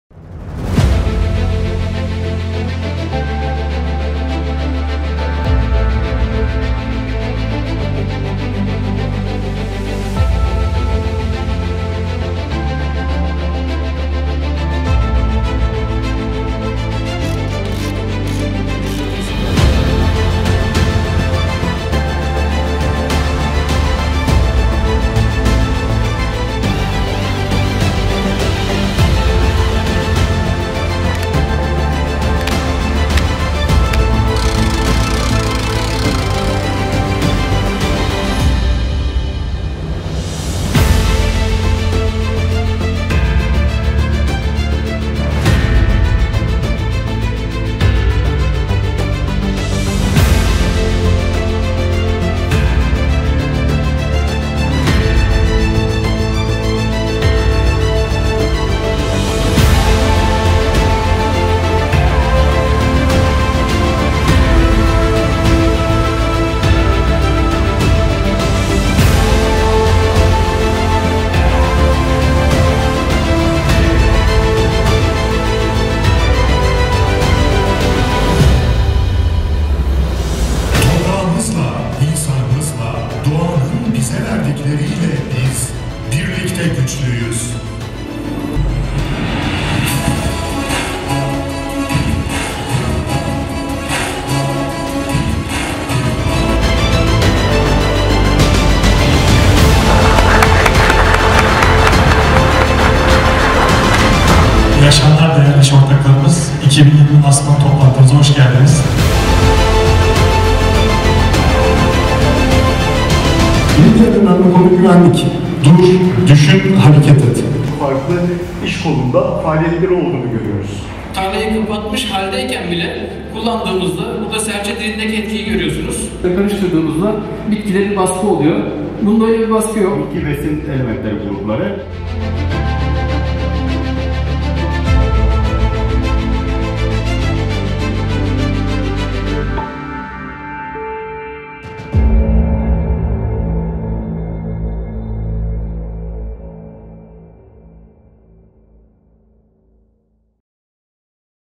Birlikte Güçlüyüz İş Ortakları ve Lansman Toplantısı | Ankara - Konya
FMC Türkiye ekibi olarak, “Birlikte Güçlüyüz İş Ortakları ve Lansman Toplantısı” kapsamında Ankara ve Konya’da bir araya geldiğimiz tüm iş ortaklarımıza teşekkür ederiz.